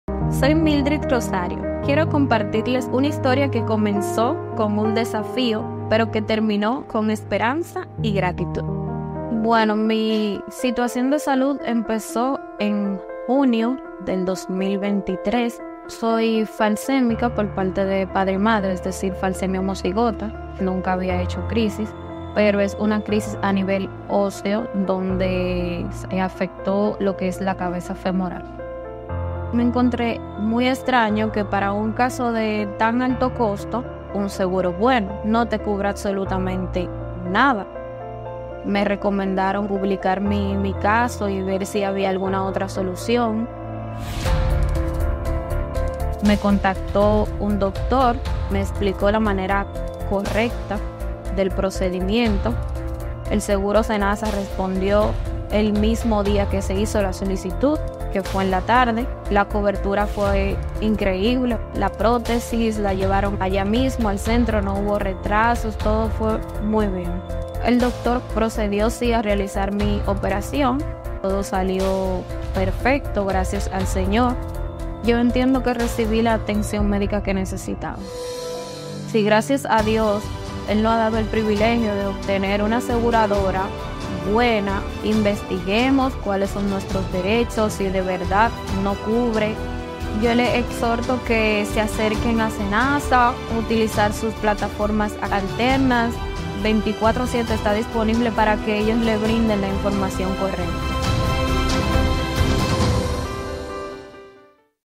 Video testimonio SeNaSa Caso Mildrid
Para nosotros siempre es un placer servirte, así como a Mildrid Rosario, quien es una de nuestras afiliadas al Régimen Contributivo, quien recibió su prótesis de acuerdo a sus derechos adquiridos.

Recuerda que para cualquier orientación puedes acercarte a nuestros Centros y Puntos de servicios, o llamarnos al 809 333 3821/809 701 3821, y desde el interior al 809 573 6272 desde el interior.